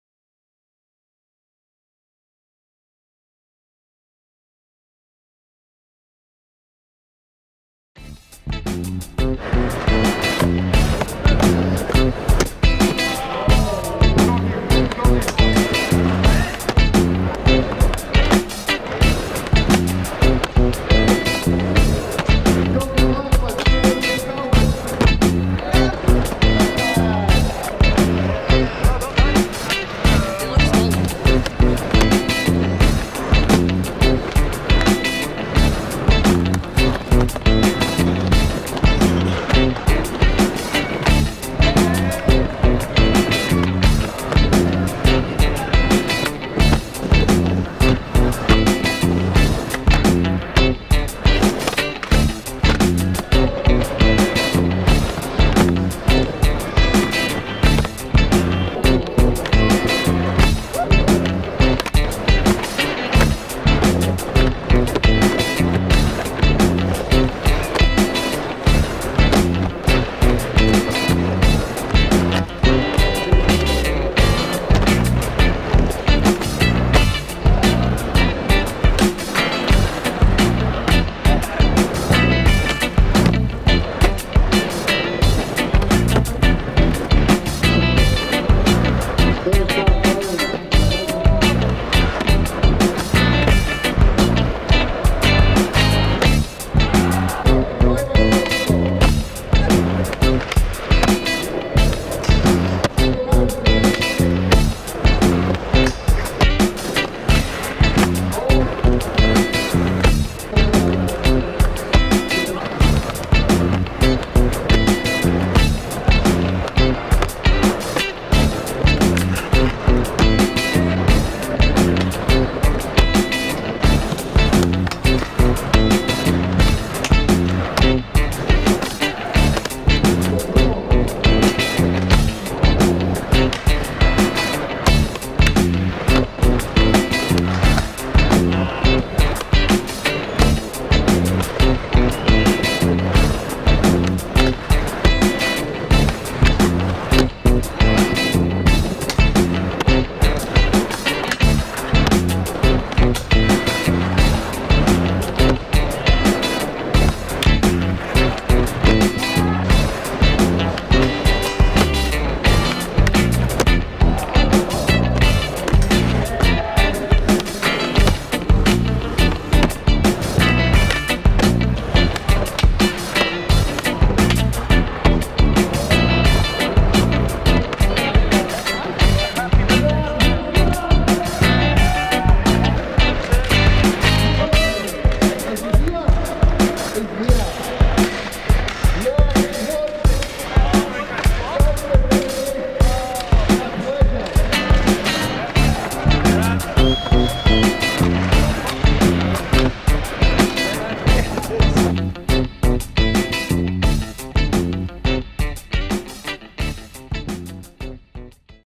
Scottish Street Champs 2015 - East Kilbride
On Sunday the 13th of December we had a Jam Session to choose a Scottish Street Skate Champion for 2015!
Standings:
1st Grant Johnson
2nd Gary Winton
3rd Jonny Paterson